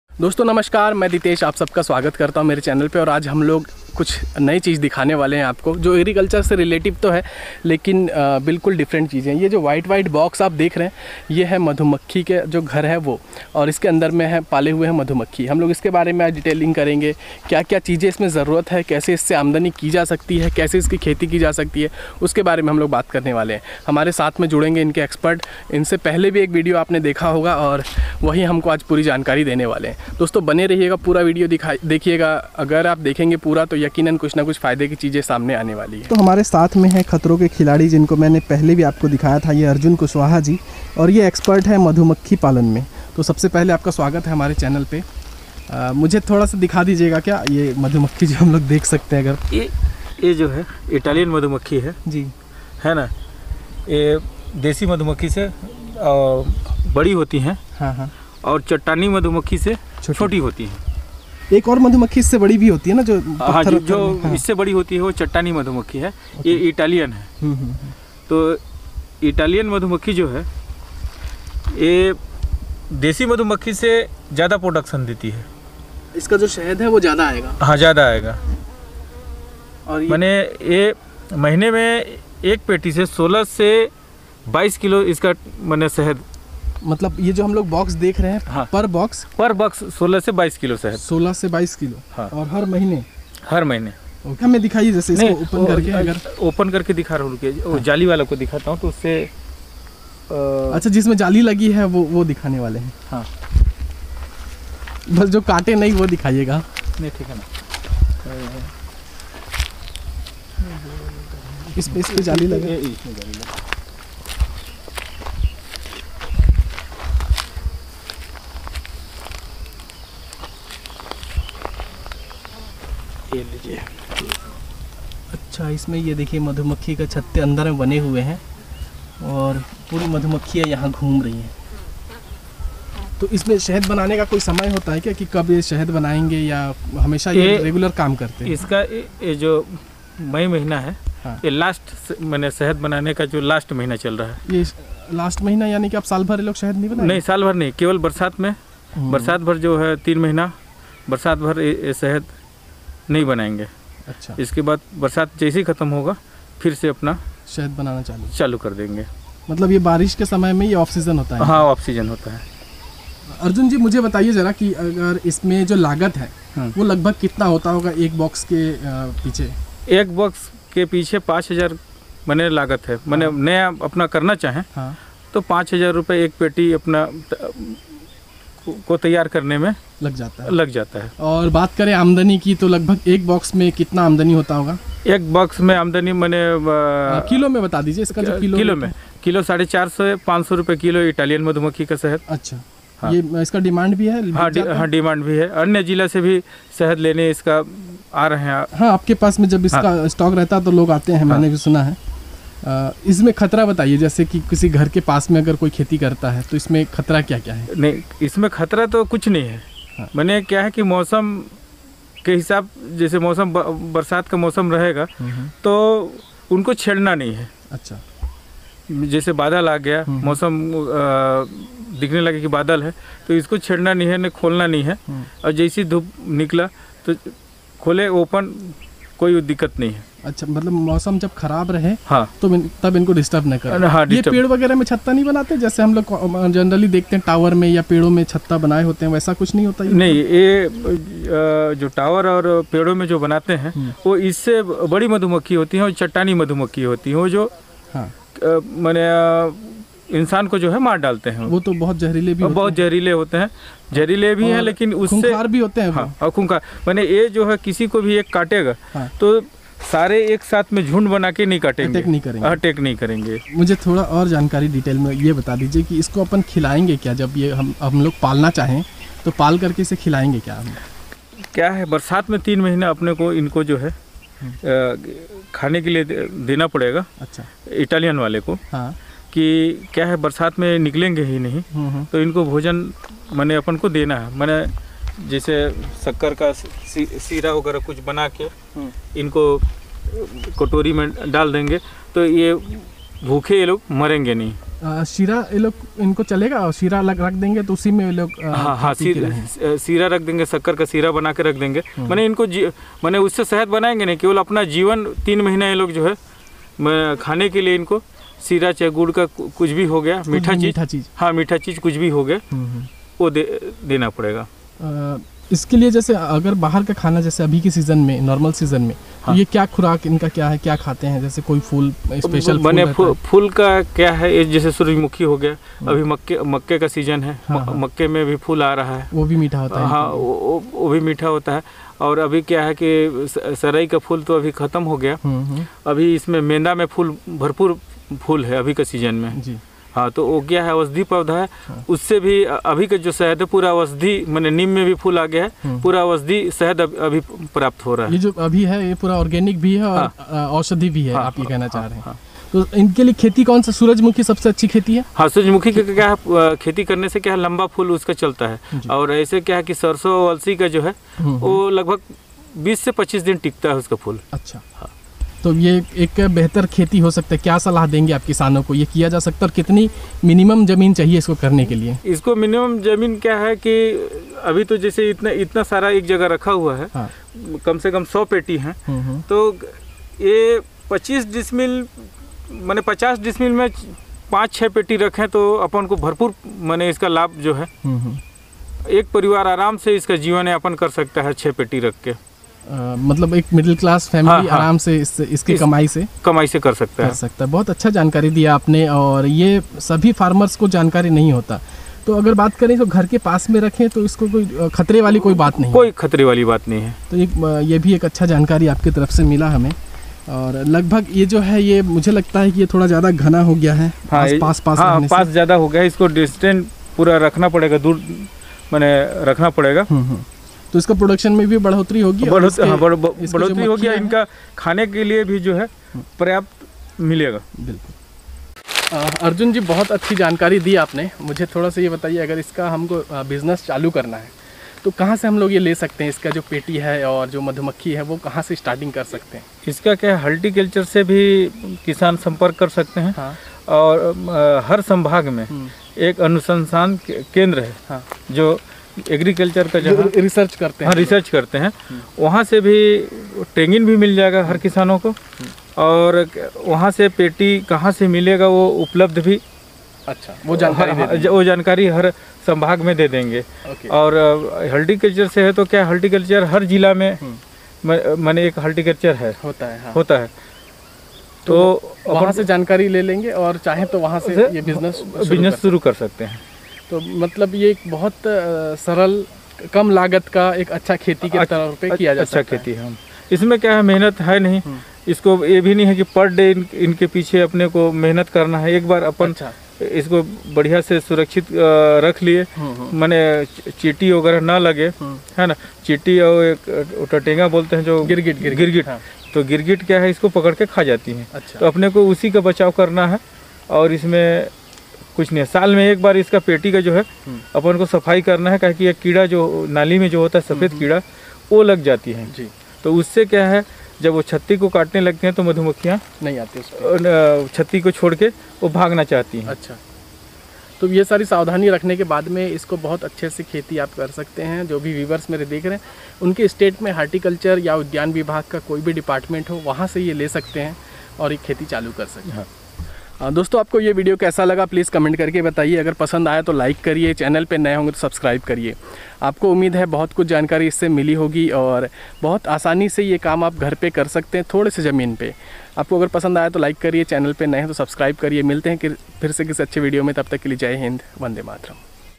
मधुमक्खी पालन // इटालियन मधुमक्खी की खेती // कम लागत की आधुनिक खेती // HoneyBee Farming //
Honey bee expert
Mr.Arjun Kushwaha, Surajpur,  chhattisgarh

Honey Bee Farming
Italian Honey Bee farming
low cost farming